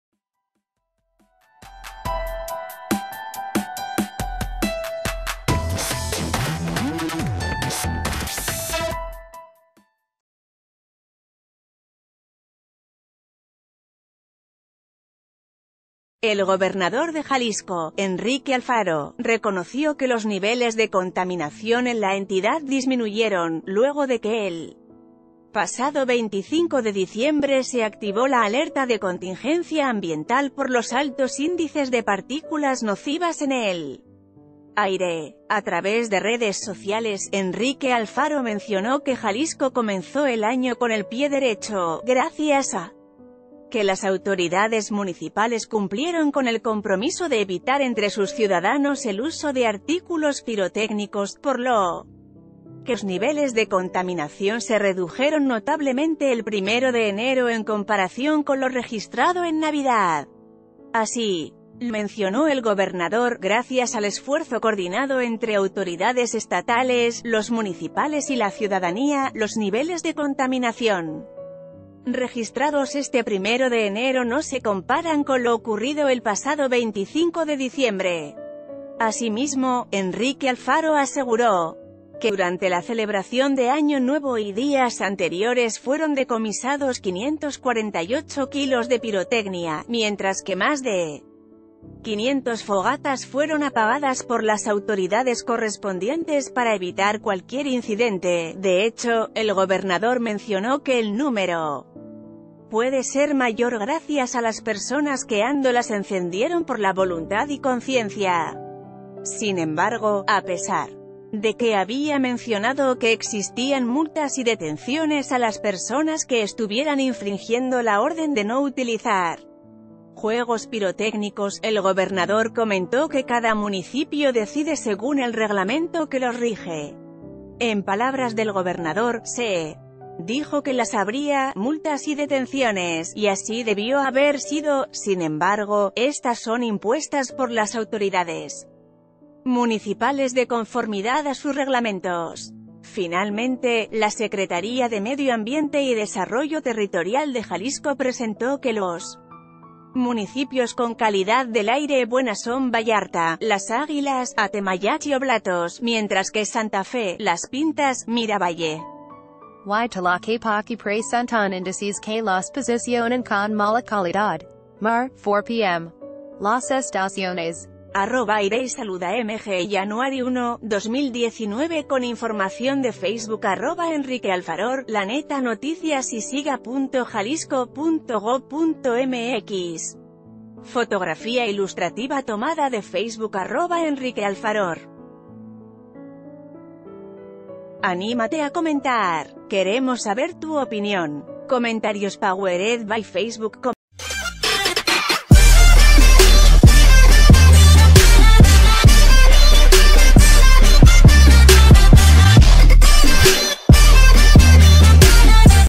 Enrique Alfaro reconoce reducción en niveles de contaminación en Jalisco
El gobernador de Jalisco, Enrique Alfaro, reconoció que los niveles de contaminación en la entidad disminuyeron, luego de que el pasado 25 de diciembre se activó la alerta de contingencia ambiental por los altos índices de partículas nocivas en el aire.